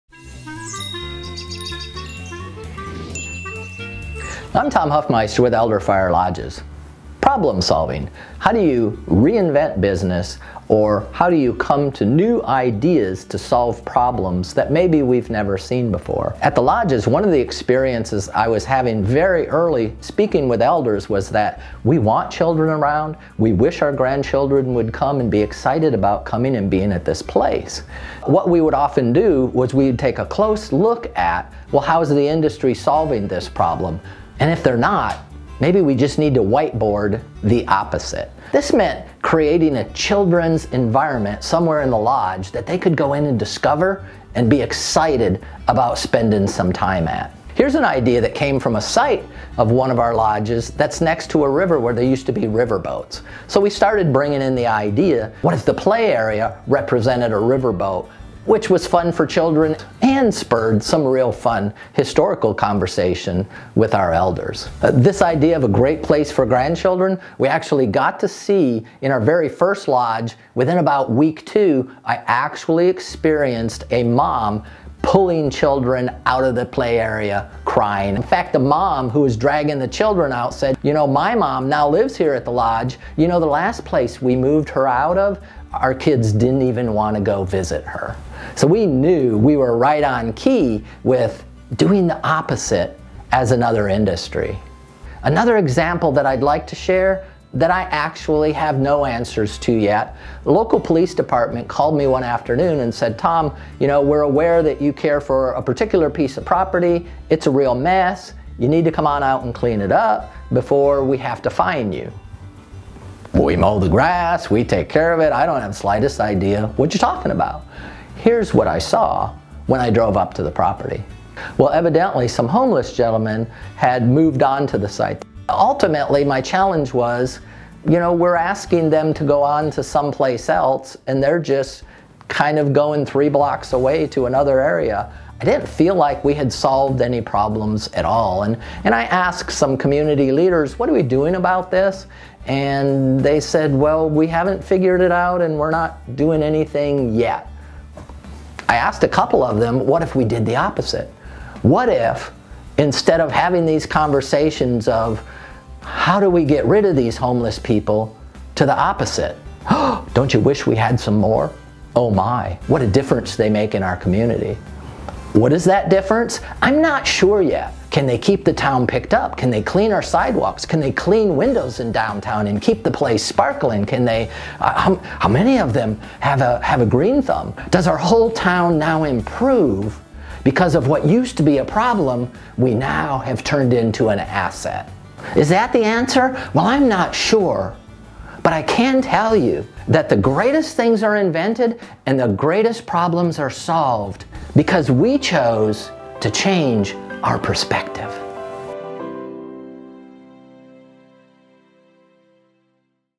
Problems, You gotta love em!
Tom Hofmeister of Elderfire Lodges discusses his problem solving techniques.